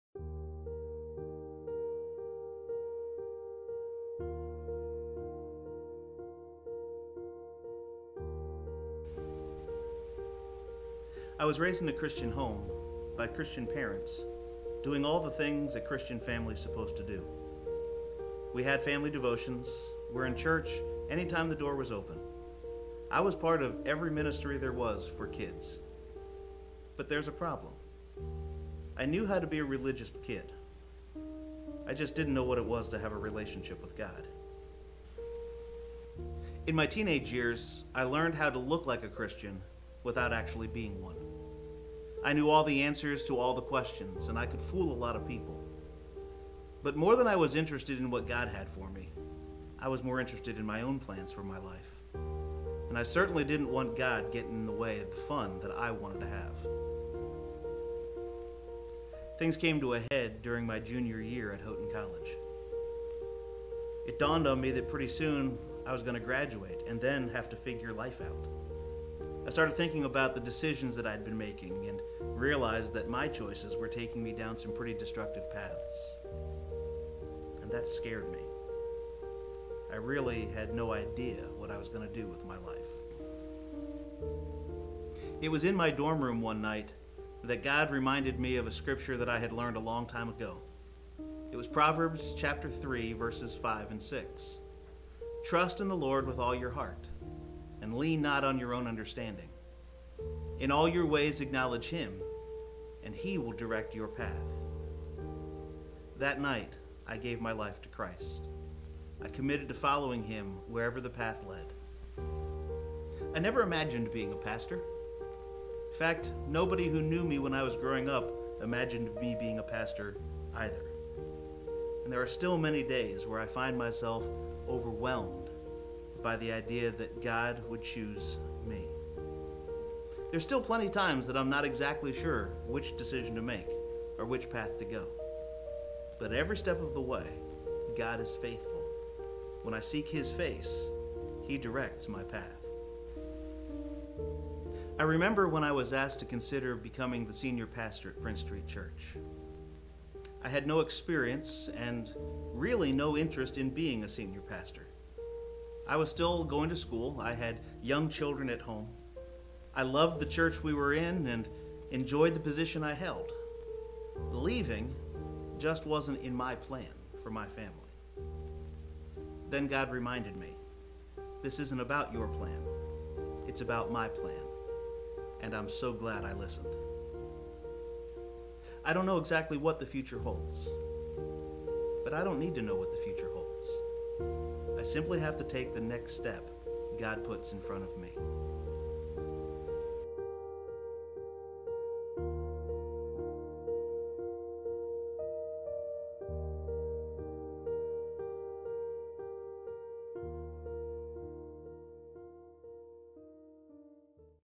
Faith Stories - Jim Bolich
Pastor Jim Bolich recalls accepting what God has planned for his life and how anything is possible through the power of God and our acceptance of his will!